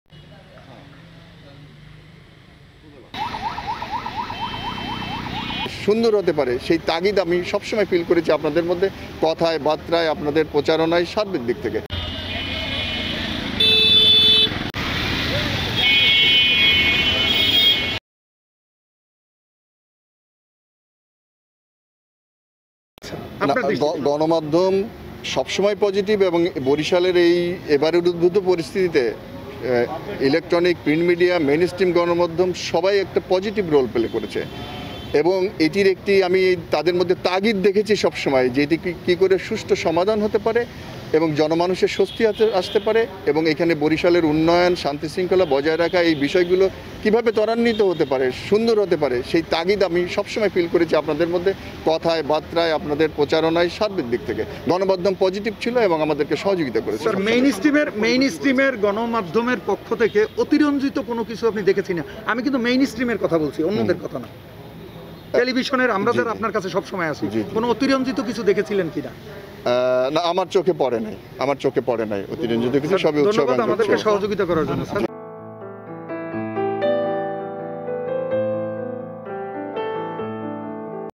যে কারনে প্রধান নিউজ চ্যানেল গুলোর প্রশংসা করলেন বরিশালের পুলিশ কমিশনার মো. শাহাবুদ্দিন খান
যে কারনে প্রধান নিউজ চ্যানেল গুলোর প্রশংসা করলেন বরিশালের পুলিশ কমিশনার